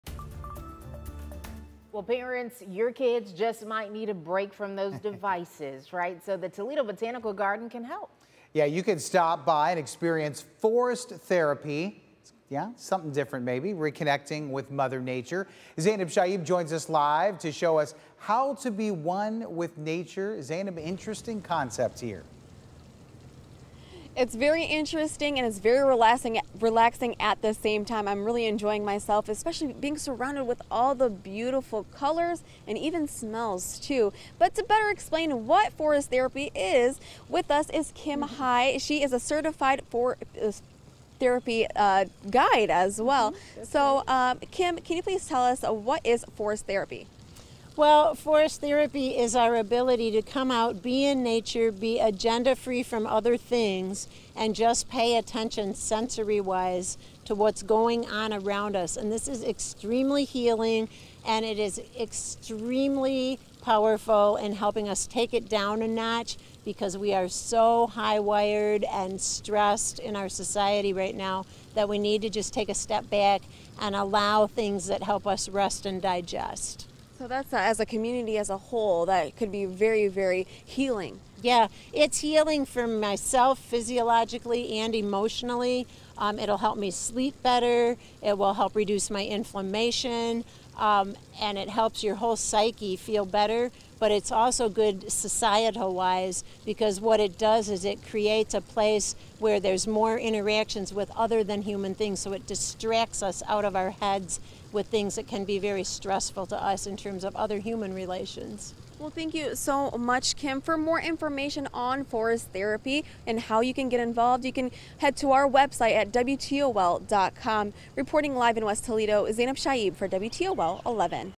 Take a break from screen time with forest therapy
Forest therapy, or "forest bathing", is a new way to relax and destress by disconnecting from your phone and spending time in nature.